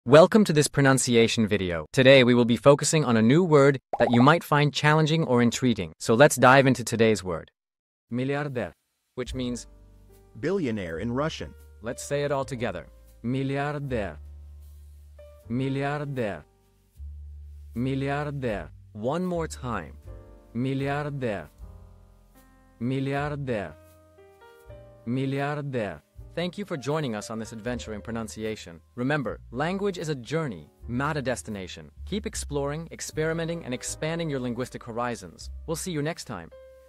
How to pronounce Milliarder | Миллиардер (Billionaire in Russian)
Master the Pronunciation of 'Milliarder | Миллиардер' - which means : Billionaire in Russian 📝 with @PronunciationRussian 🎓🗣️ - [Your Guide to Russian]

Learn to pronounce 'Milliarder' perfectly in Russian with our easy and engaging tutorial by @PronunciationRussian! 📖📽️ In this comprehensive video, we provide you with native speaker examples 🗣️👂 and phonetic breakdowns 🎵 to ensure you get the pronunciation just right.